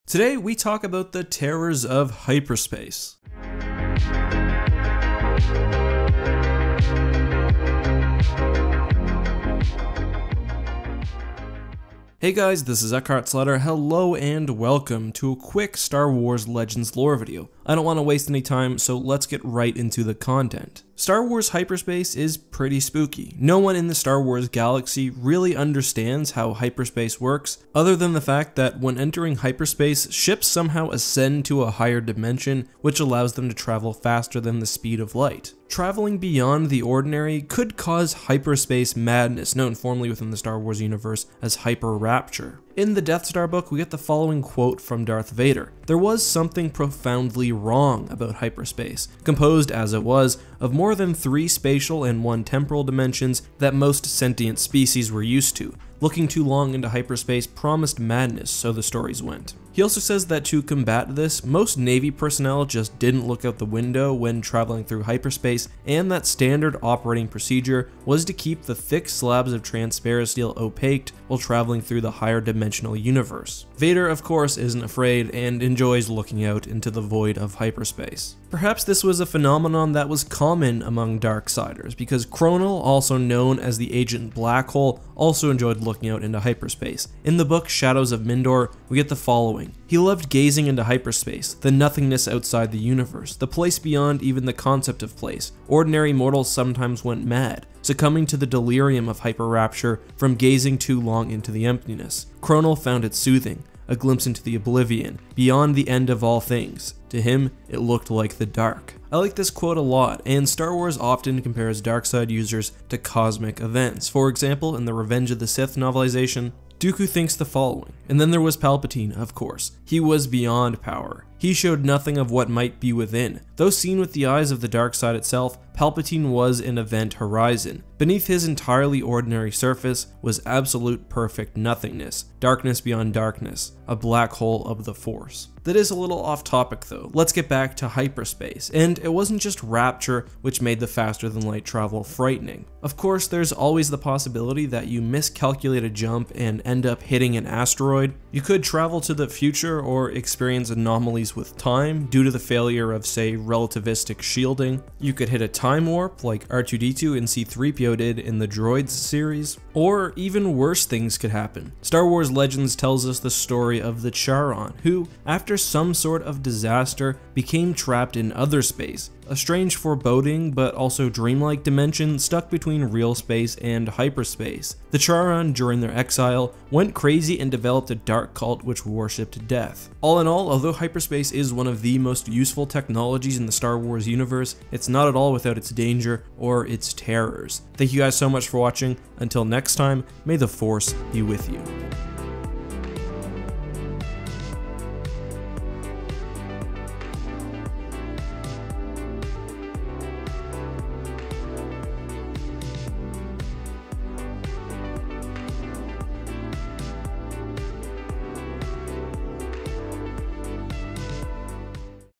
The HORRORS of HYPERSPACE | Star Wars Legends Lore
On today's Star Wars Legends Lore video, we look at the horrors of hyperspace.

From the Otherspace, to time travel, to hyperspace madness (hyper rapture), I cover the Horrors of Hyperspace on today's Star Wars Legends Lore video.